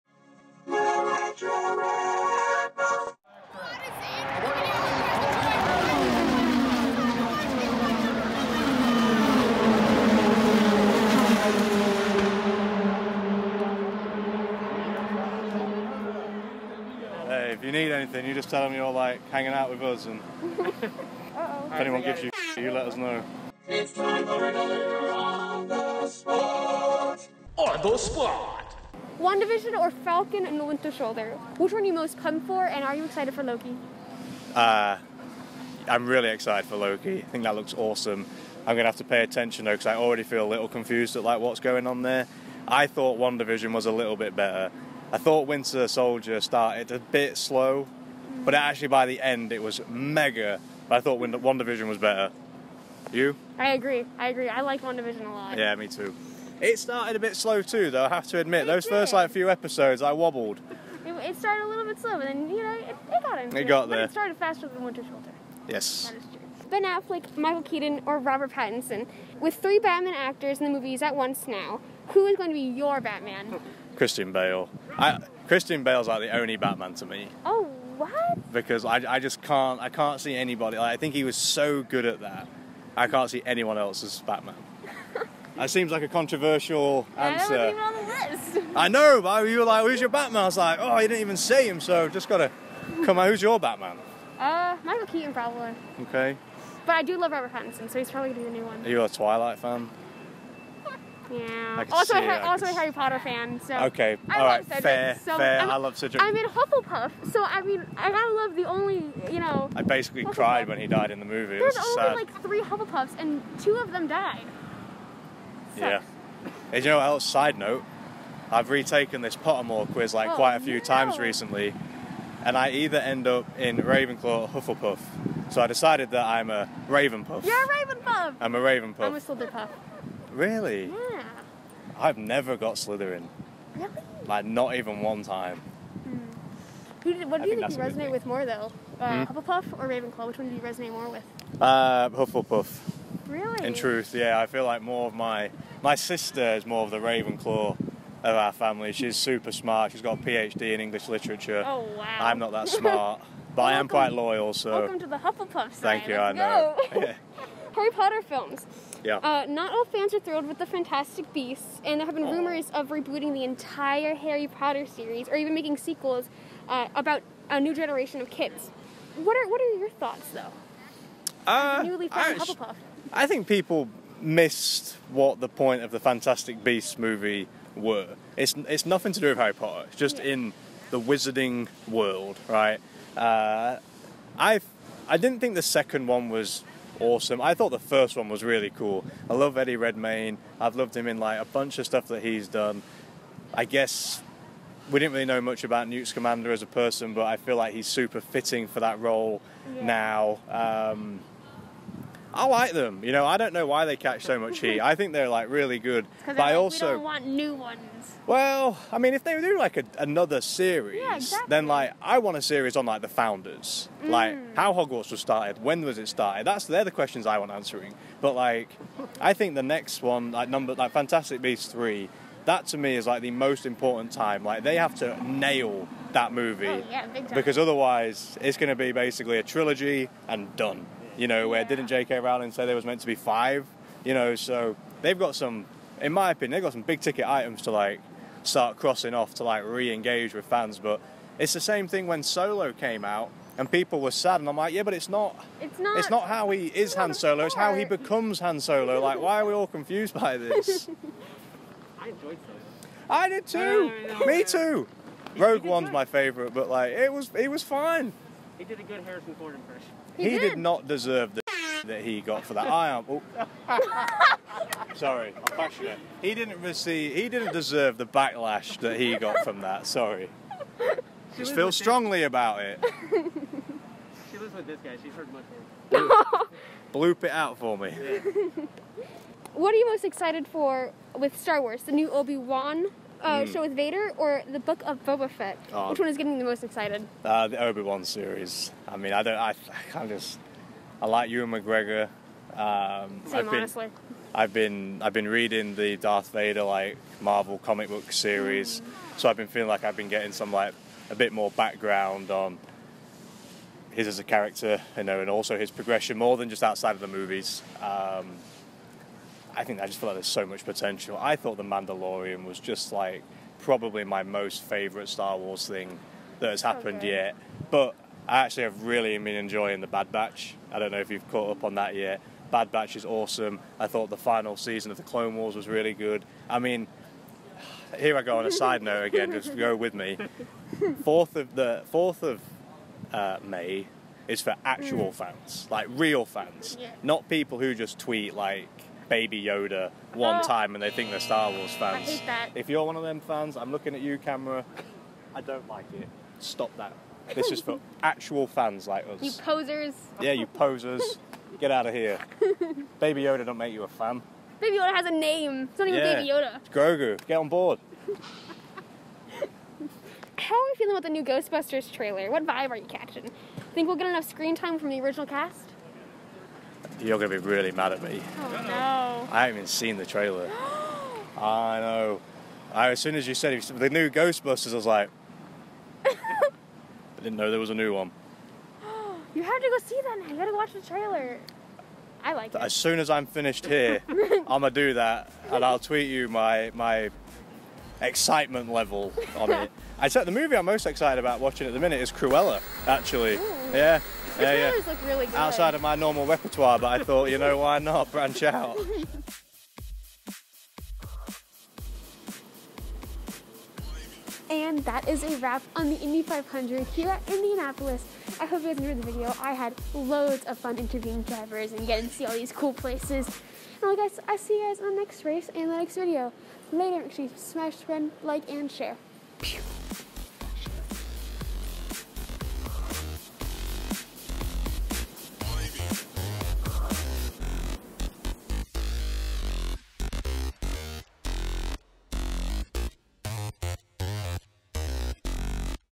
"ON THE SPOT" W/JACK HARVEY
Hey guys! Travel along with me to IMS for the 2021 Indy500 MediaDay! I manage to find MeyerShankRacing IndyCar driver JackHarvey after his photoshoot for a quick chat about everything Nerds love besides racing!

We find some time at the end of the day to chat about the world of Marvel, Batman, HarryPotter, FantasticBeasts HanSolo, StarWars, GhostbustersAfterlife & everything else in between!

Stay in the know with me! Join the Retro Pop-Culture Rebellion!

Much love to all my present & future Rebels!